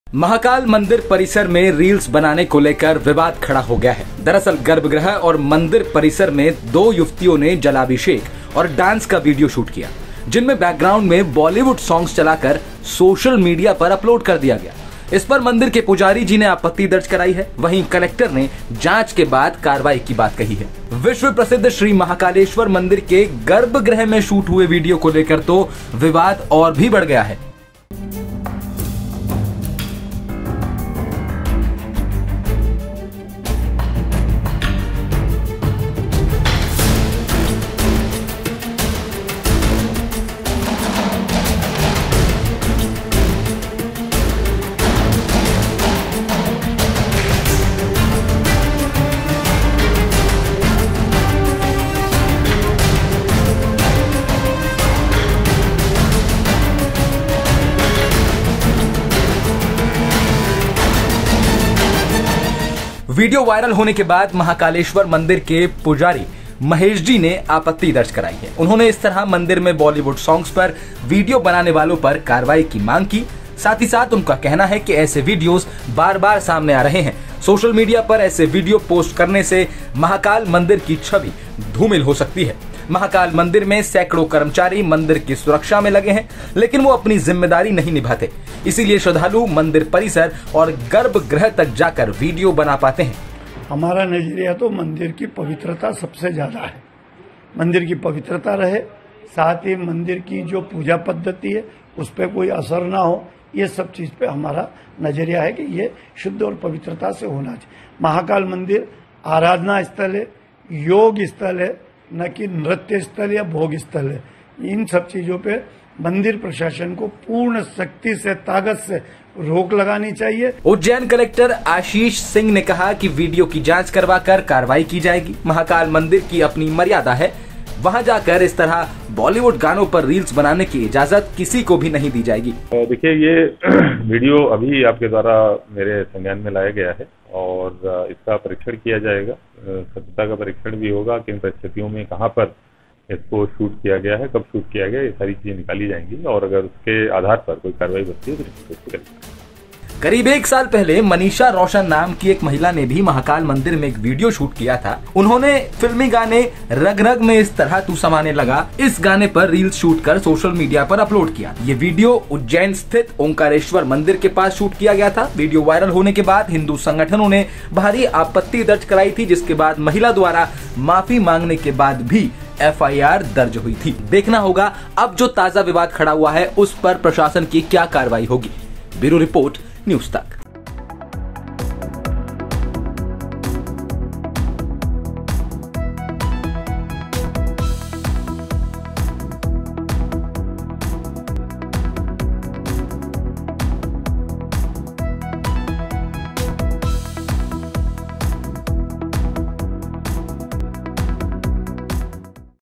Ujjain: महाकाल मंदिर में Reels बनाने पर गुस्साएं पुजारी, शिकायत दर्ज कर होगी कार्यवाही।
महाकाल मंदिर परिसर में रीलस बनाने को लेकर विवाद खड़ा हो गया है। दरअसल, गर्भगृह और मंदिर परिसर में दो युवतियों ने जलाभिषेक और डांस का वीडियो शूट किया जिनमें बैकग्राउंड में बॉलीवुड सॉन्गस चलाकर सोशल मीडिया पर अपलोड किया गया। इस पर मंदिर के पुजारी जी ने आपत्ति दर्ज कराई है। वहीं, कलेक्टर ने जांच के बाद कार्रवाई की बात कही है।